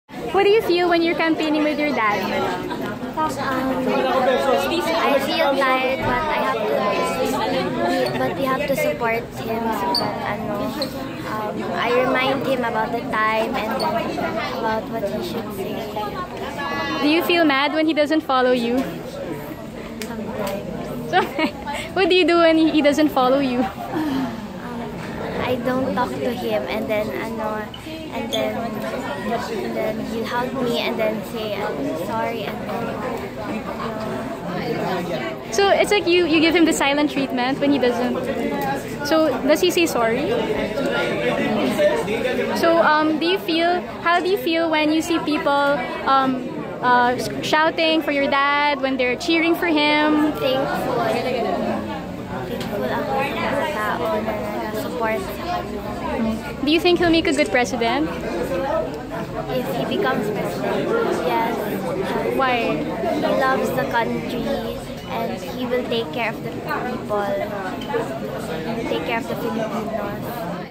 A one minute chat with Kitty Duterte
More videos on Rappler Follow Rappler for the latest news in the Philippines and across the globe. Meet Rodrigo Duterte's youngest daughter Kitty, his constant companion on the campaign trail.